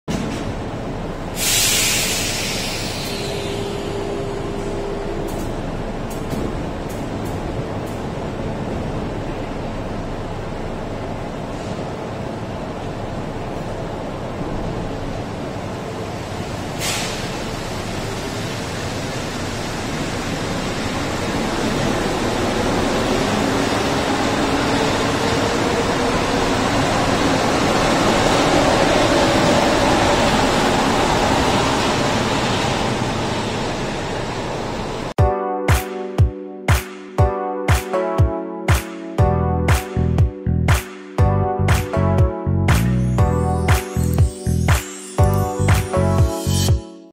Concarril NM 83A-B (M.468/M.486) 4K UHD
25 Julio 2020

Este Tren Tiene La Particularidad De Tener Una Motriz De Modelo A y Una De Modelo B, Amabas De NM 83.